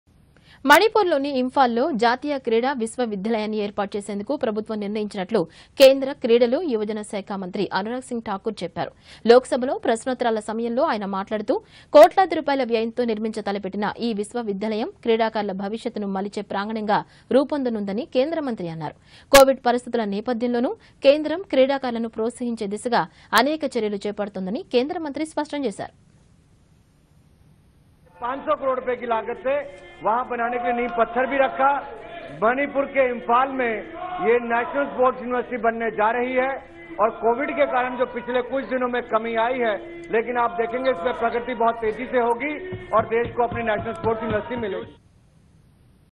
🔴మణిపూర్ లోని జాతీయ క్రీడా విశ్వవిద్యాలయం ఏర్పాటుకు కేంద్రం నిర్ణయం:కేంద్రమంత్రిఅనురాగ్ సింగ్ ఠాకూర్
మణిపూర్ లోని ఇంఫాల్ లో జాతీయ క్రీడా విశ్వ విద్యాలయాన్ని ఏర్పాటు చేసేందుకు ప్రభుత్వం నిర్ణయించినట్లు  కేంద్ర, క్రీడలు, యువజన శాఖా మంత్రి అనురాగ్  సింగ్ ఠాకూర్  చెప్పారు. లోక్ సభలో ప్రశ్నోత్తరాల సమయంలో ఆయన మాట్లాడుతూ... కోట్లాది రూపాయల వ్యయంతో నిర్మించ తలపెట్టిన ఈ విశ్వవిద్యాలయం క్రీడాకారుల భవిష్యత్ ను మలిచే ప్రాంగణంగా రూపొందనుందని కేంద్రమంత్రి అన్నారు. కొవిడ్ పరిస్ధితుల నేపథ్యంలోనూ కేంద్రం క్రీడాకారులను ప్రోత్సహించే దిశగా అనేక చర్యలు చేపడుతోందని కేంద్రమంత్రి స్పష్టం చేశారు.